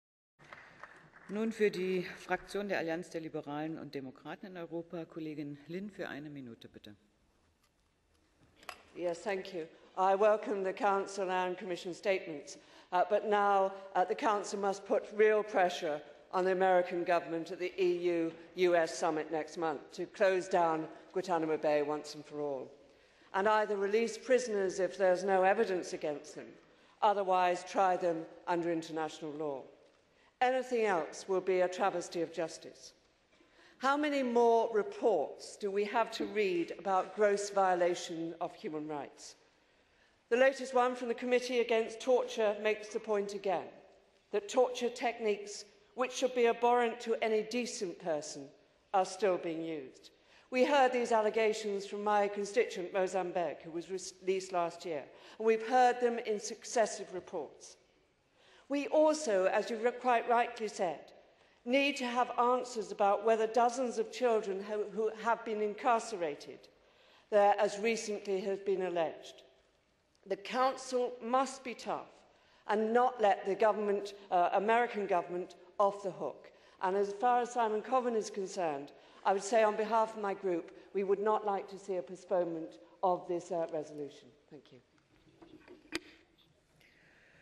Liz Lynne on Situation of the prisoners in Guantánamo
Speech by Liz Lynne MEP on :
Situation of the prisoners in Guantánamo